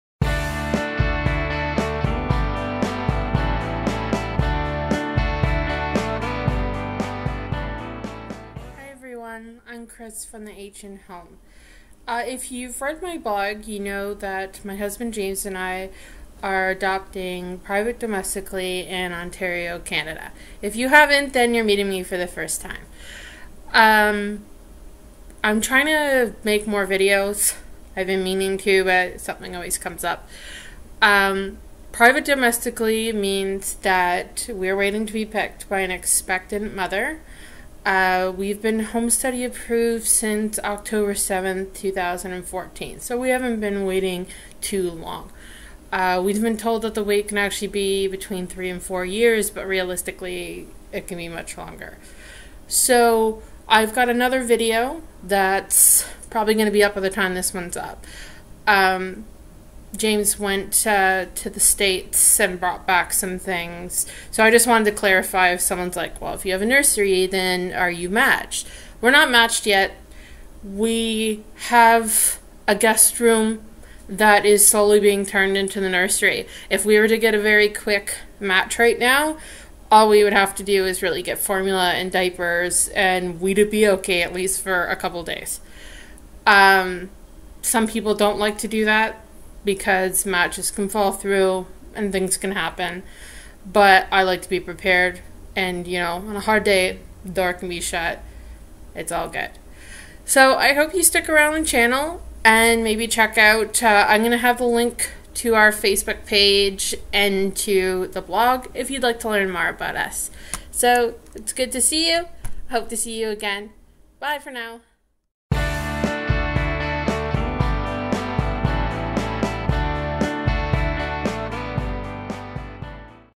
A Quick Introduction
Hi There!  I'm Kris, of The H in Home.  My husband James and I are waiting to adopt in Ontario, Canada.  This is just a very brief intro talking about us adopting and why we have baby things while we're still waiting for a match.